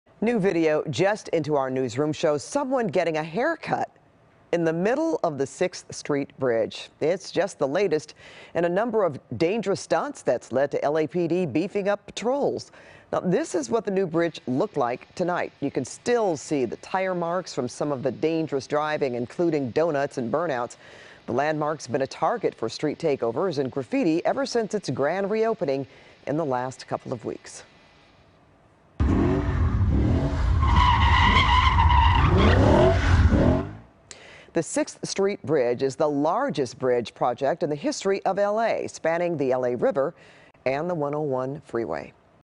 Man gets haircut in middle of new 6th Street Bridge
A man was captured on video getting a haircut in the middle of the 6th Street Bridge, the latest in a string of stunts that have taken place on the newly opened viaduct bridge.